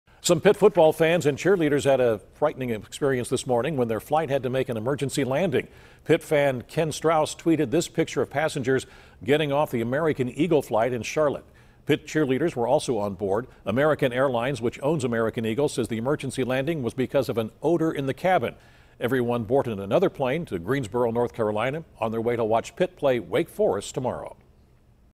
Plane Carrying Pitt Fans, Cheerleaders Makes Emergency Landing
Some Pitt Panthers fans and cheerleaders were on board on a plane that had to make an emergency landing Friday morning.